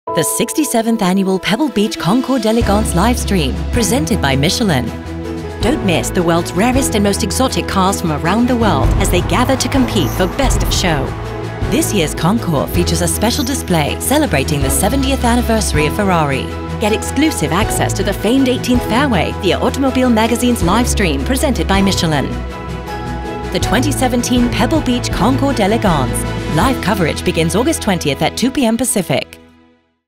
Exclusive Coverage of the 67th Annual Pebble Beach Concours d’Elegance Live Stream
Get exclusive access to the 67th Annual Pebble Beach Concours d’Elegance via Automobile Magazine’s live stream, presented by Michelin. Live coverage begins August 20 at 2PM PST at AutomobileMag.com/PebbleBeachConcours. For more details